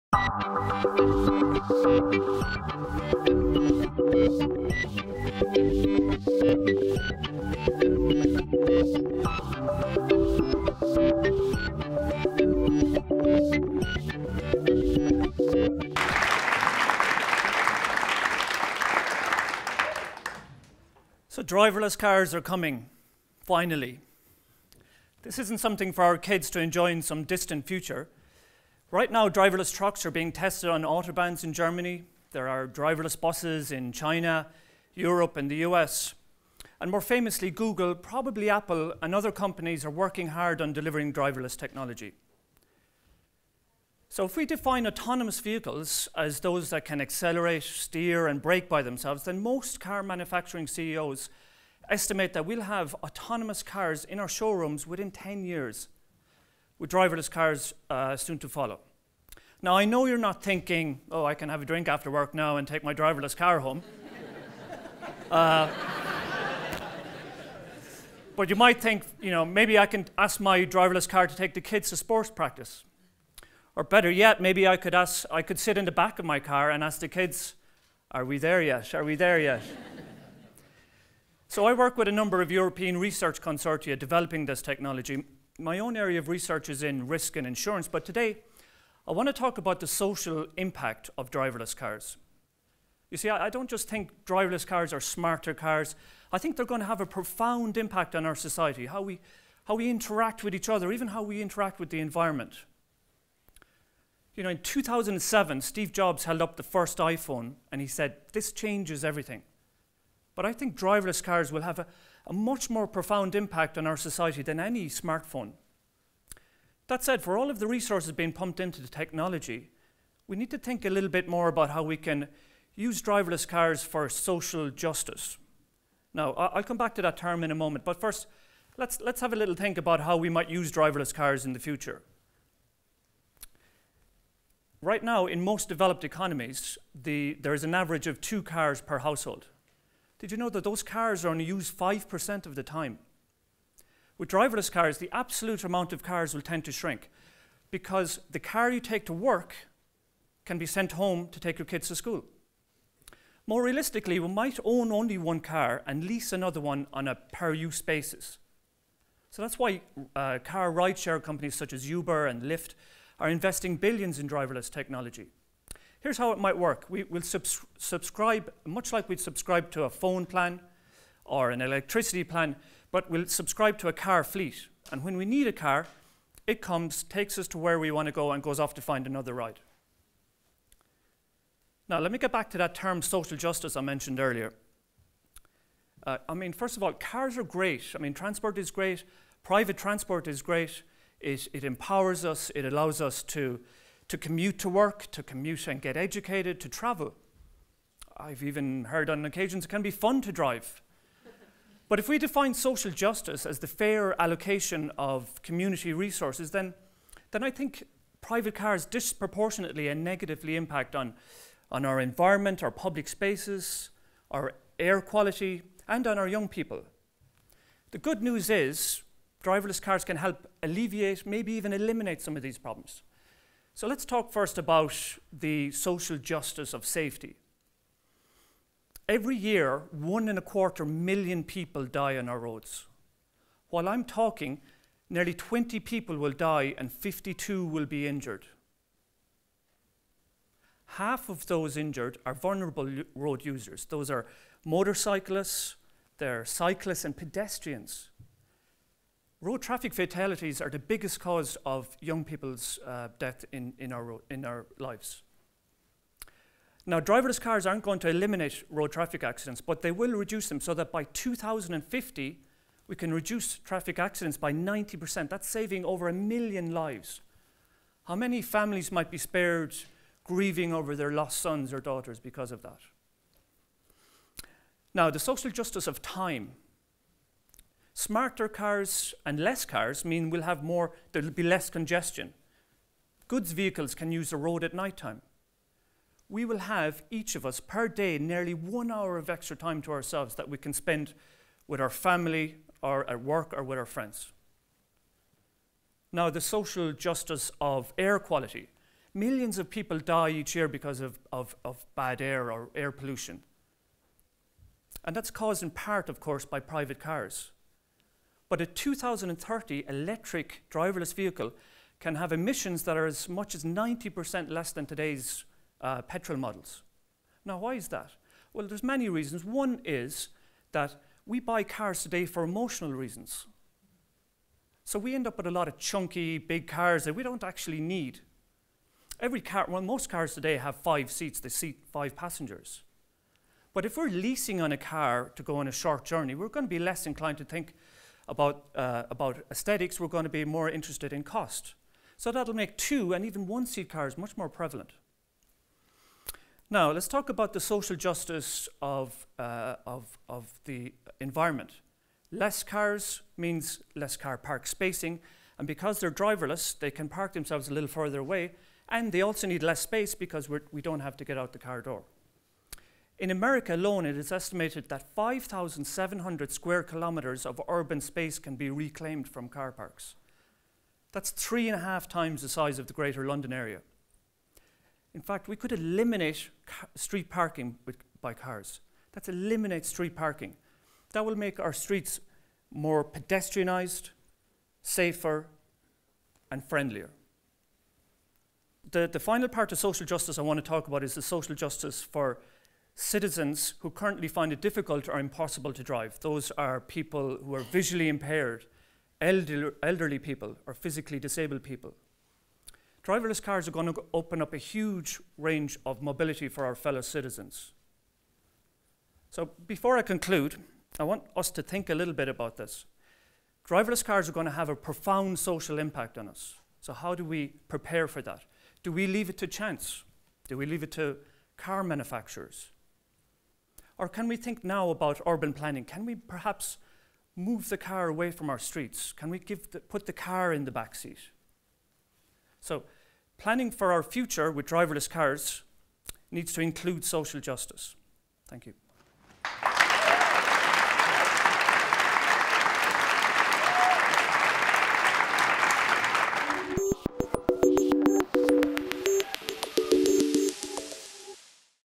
Driverless Cars – The Road Ahead | Finbarr Murphy | TEDxFulbrightDublin
During this talk, Dr. Finbarr Murphy discusses the profound social and economic implications of autonomous vehicles.

Dr. Finbarr Murphy is a Lecturer in Finance in the Kemmy Busisness School in the University of Limerick. Before taking up his position in UL, Finbarr was a Vice President of Convertible Bond Trading with Merrill Lynch London. Instruments traded included CBs, equities, equity derivatives and US Treasury Futures contracts to actively manage complex hedging strategies. Finbarr was a Fulbright Awardee in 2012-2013.